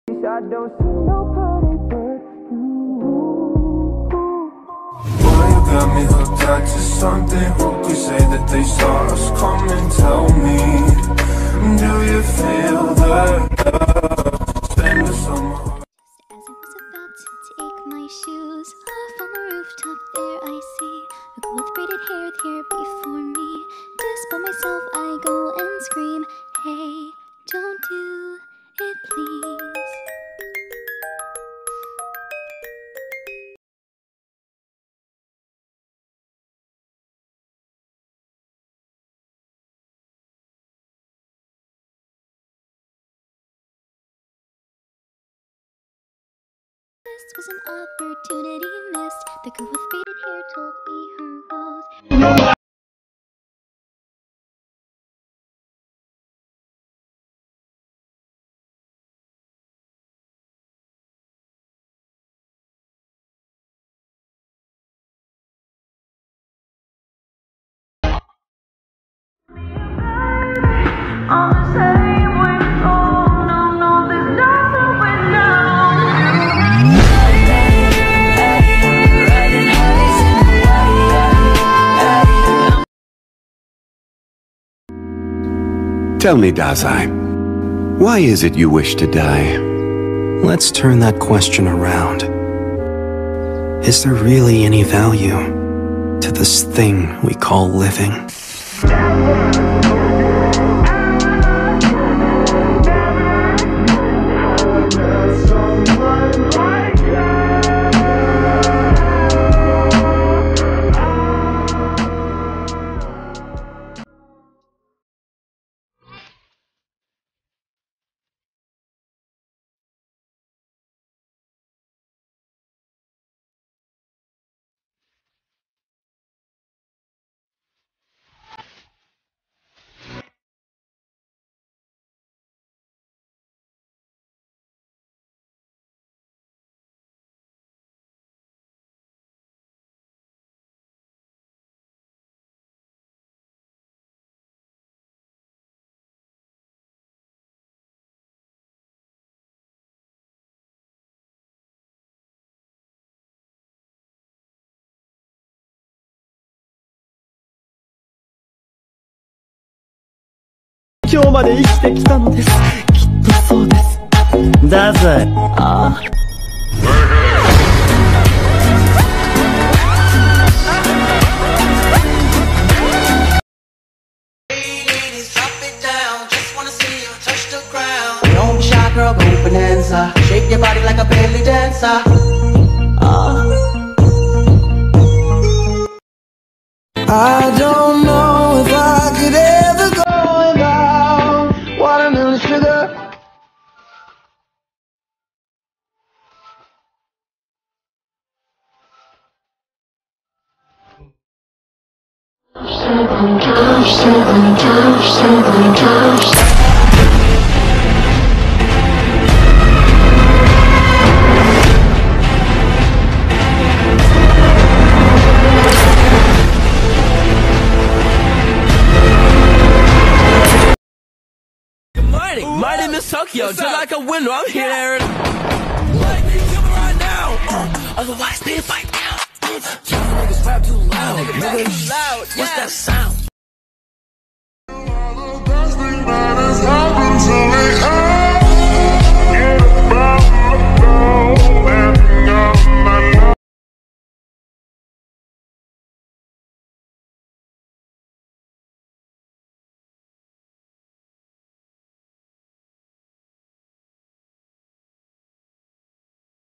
three deities react to takemichi as dazai + izana and chifuyu || no part 2 || late post:)
I'm really tired:)